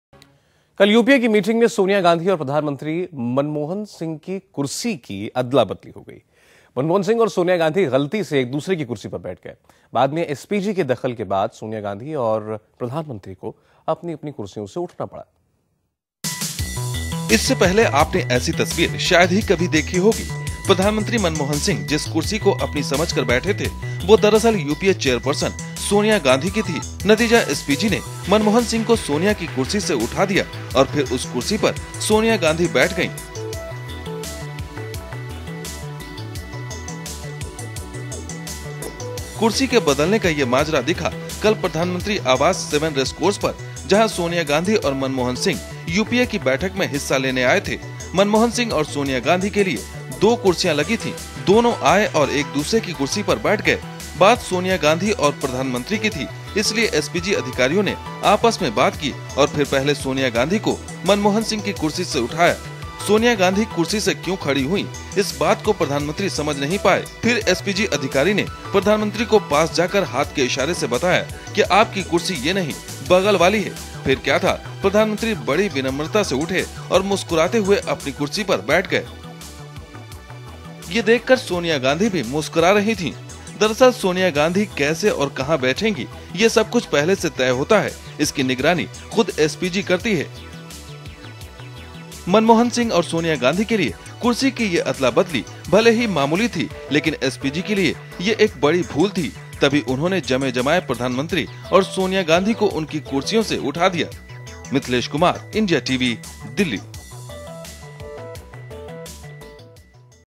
Kissa Kursi Ka : PM, Sonia Exchange Seats At UPA Meeting
It was not a case of musical chairs, but the Prime Minister Dr Manmohan Singh and UPA chairperson Sonia Gandhi had to exchange their seats on Tuesday evening at 7, Race Course Road, the Prime Minister's residence.